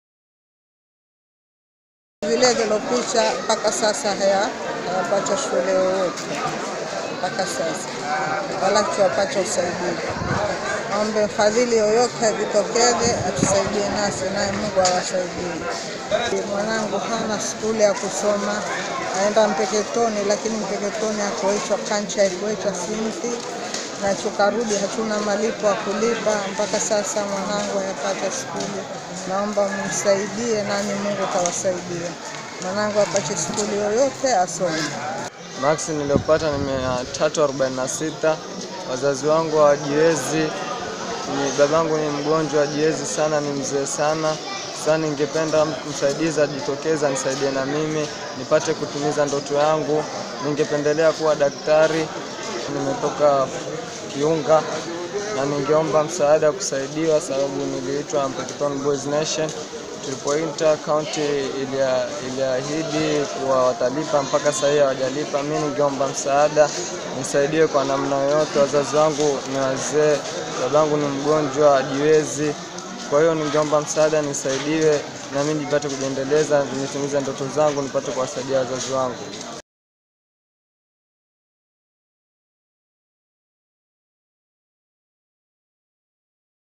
Lamu boy seeks school fees to join form one
A family in terror prone Kiunga area in Lamu East is seeking financial assistance, to enable their son join form one and pursue his secondary school education.